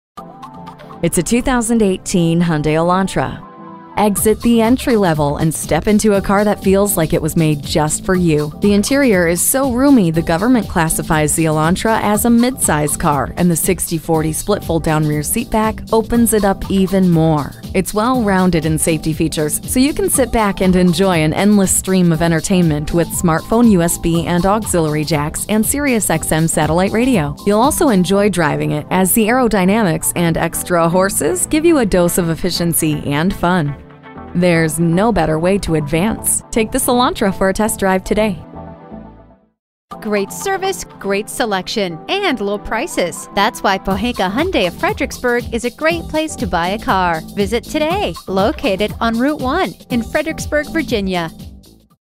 New 2018 Hyundai Elantra Fredericksburg VA Richmond, VA HJH311443 - SOLD
Year: 2018
Make: Hyundai
Model: Elantra
Trim: Value Edition
Engine: 2.0L 4-Cylinder
Transmission: Automatic
Color: Silver
Interior: Black
Stock #: HJH311443
VIN: 5NPD84LF1JH311443
Silver 2018 Hyundai Elantra Value Edition FWD Automatic 2.0L 4-CylinderbrbrRecent Arrival! 37/28 Highway/City MPG

Address:
5200 Jefferson Davis Hwy